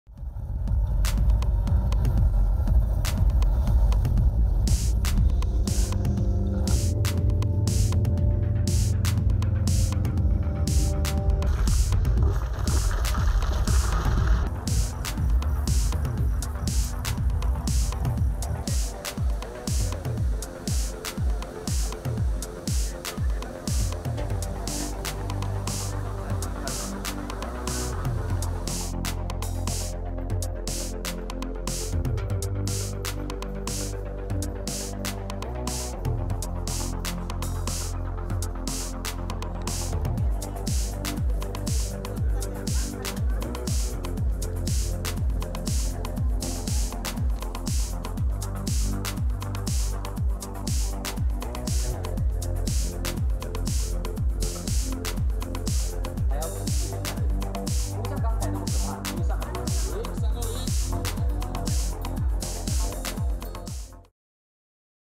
Lights galore as Taiwan holds arts festival | AFP
People gather in the streets to enjoy Nuit Blanche, an international all-night arts festival in Taipei with a hundred artists creating 20 art installations and holding 50 performances.